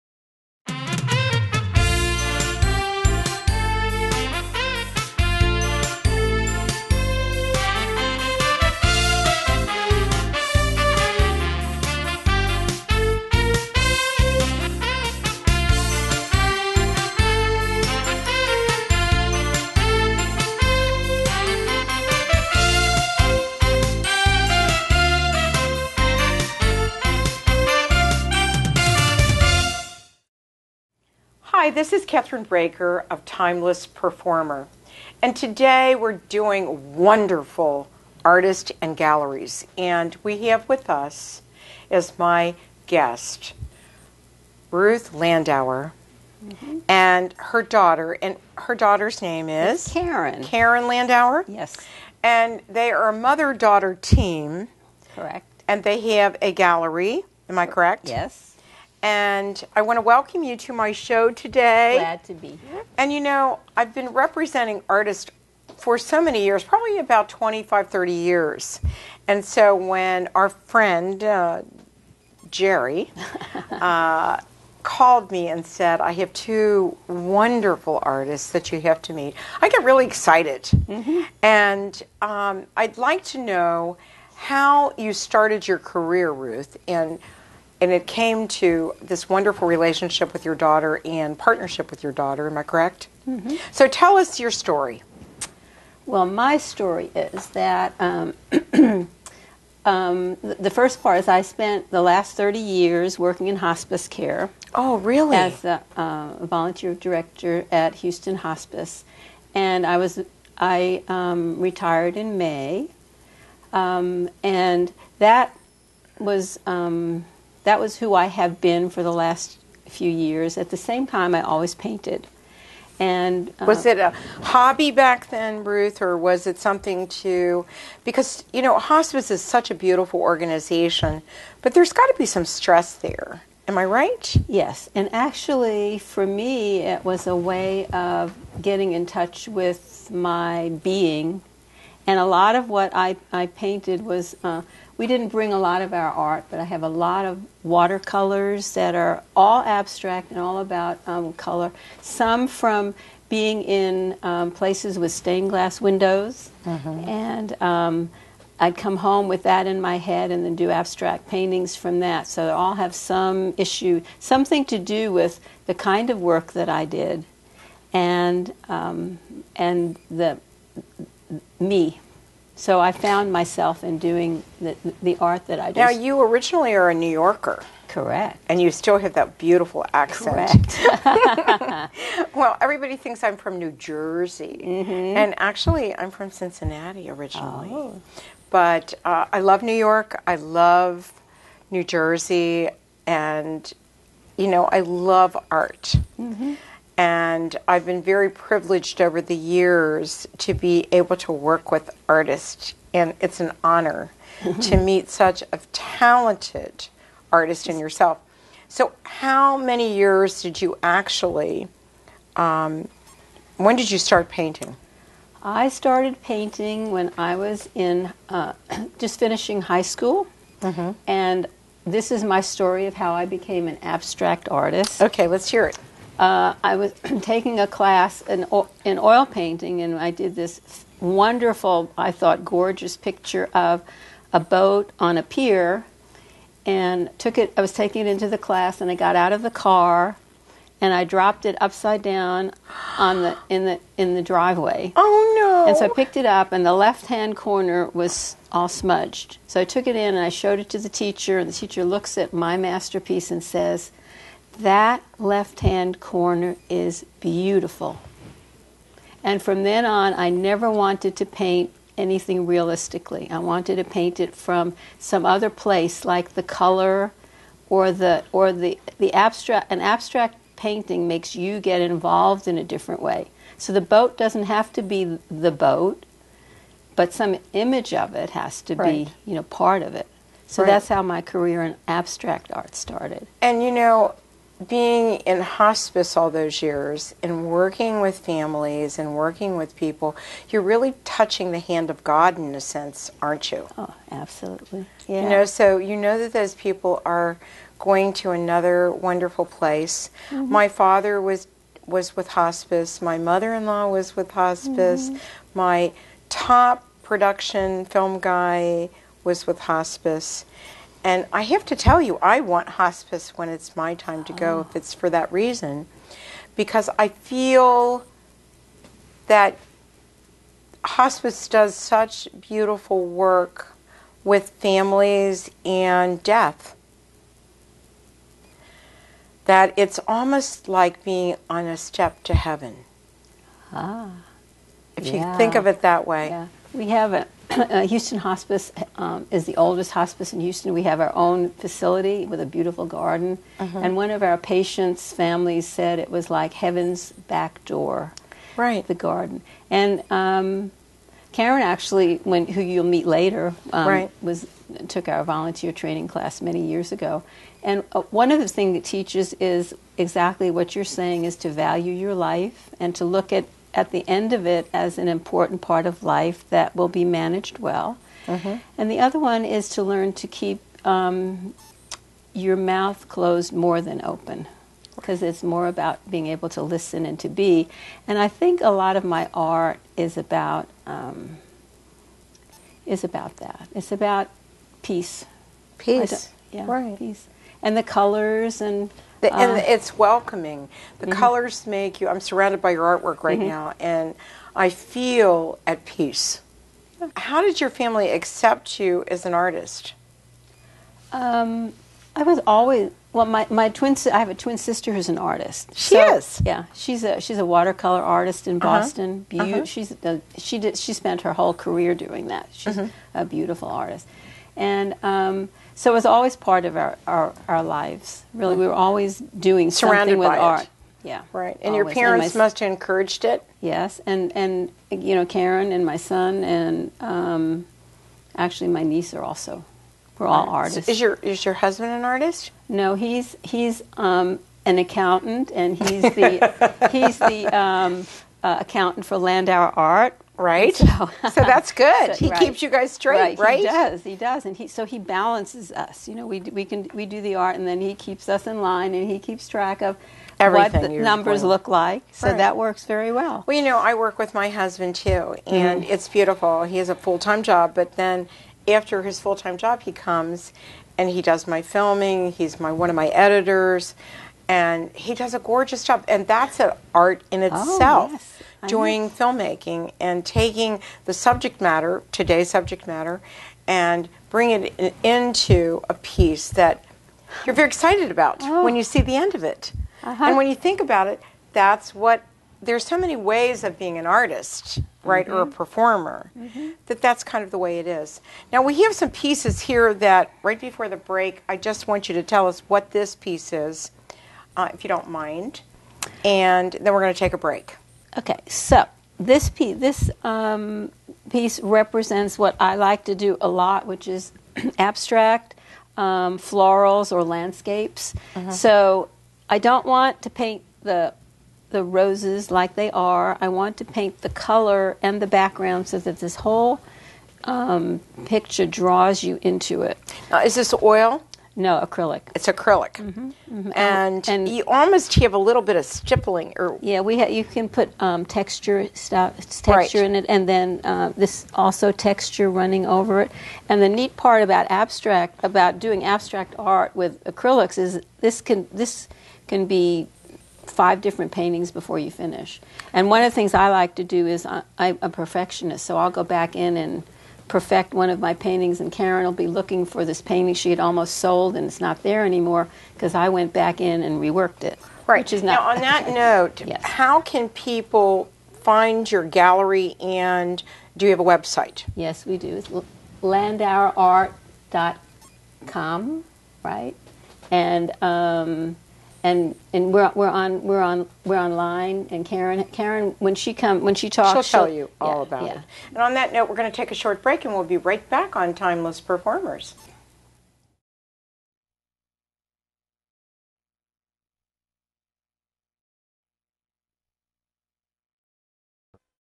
“Born B-4 64” Kathryn discusses Art with Timeless Performers Ruth and Caryn Landauer - Segment 1
Kathryn Raaker discusses art with Ruth Landauer and her daughter, Caryn Landauer, of Landauer Art, Houston, Texas. They discuss the different styles of painting and will review some different pieces of Art. This Mother – Daughter team have many wonderful pieces of art to display at their Studio.